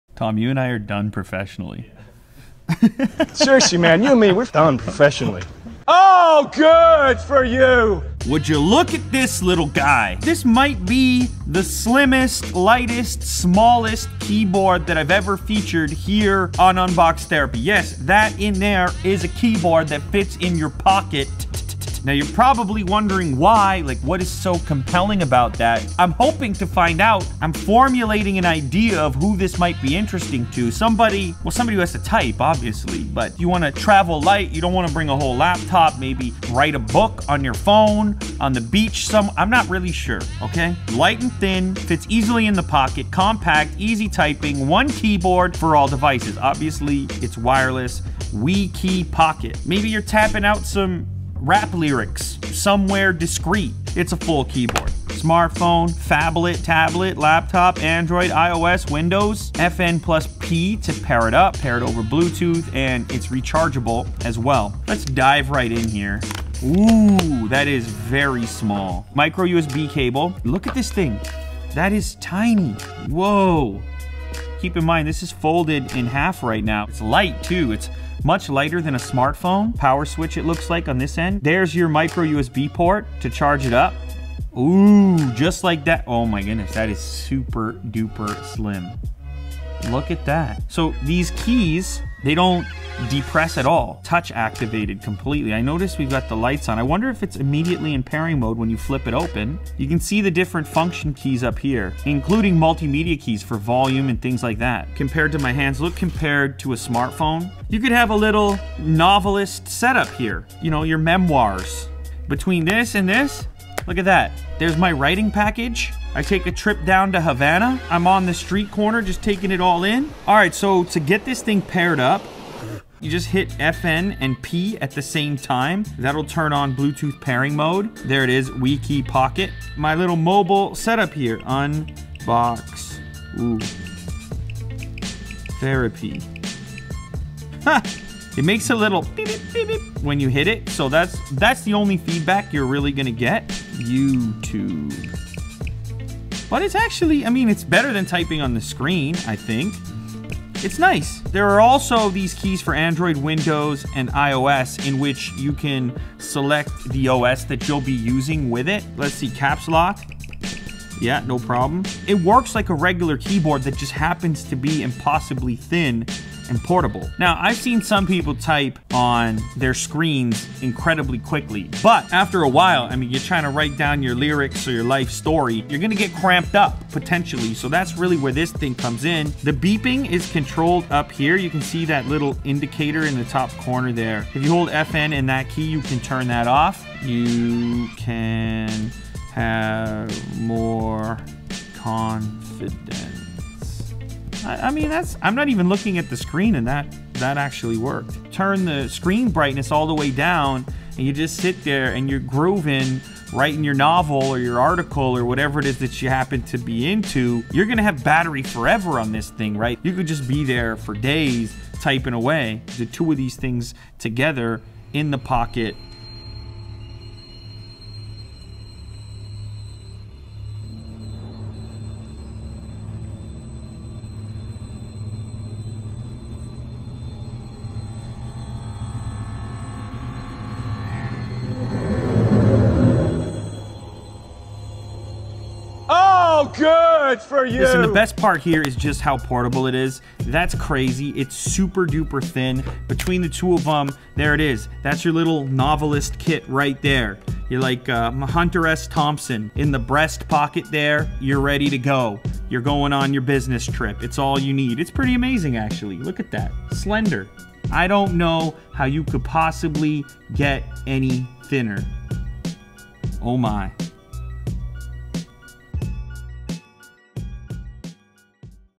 Unboxing The World's Thinnest Keyboard
Wekey Pocket Keyboard (USA Link)
Wekey Pocket Keyboard (International)

The Wekey pocket keyboard is an incredibly thin and light keyboard for mobile devices. It folds in half for easy packing and uses bluetooth to connect wirelessly.